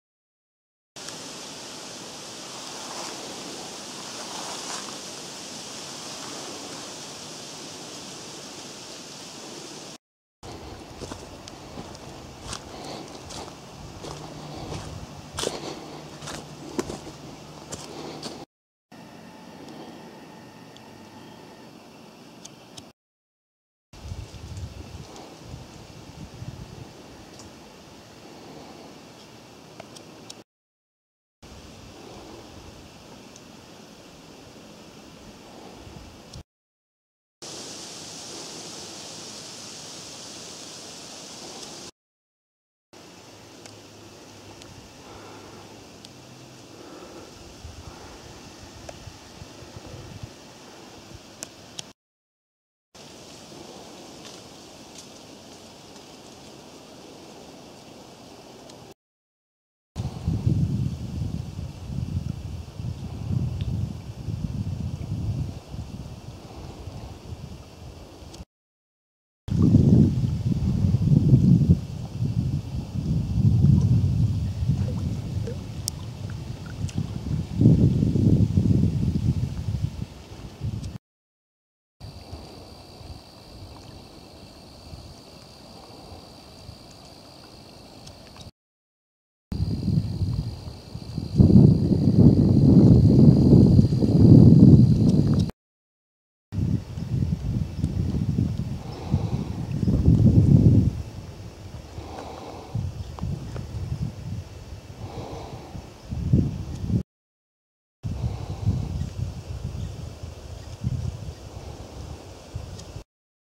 Hike to Lake Agnes from Caribou Lake, Lutsen, Minnesota
short hike from Caribou Lake to Lake Agnes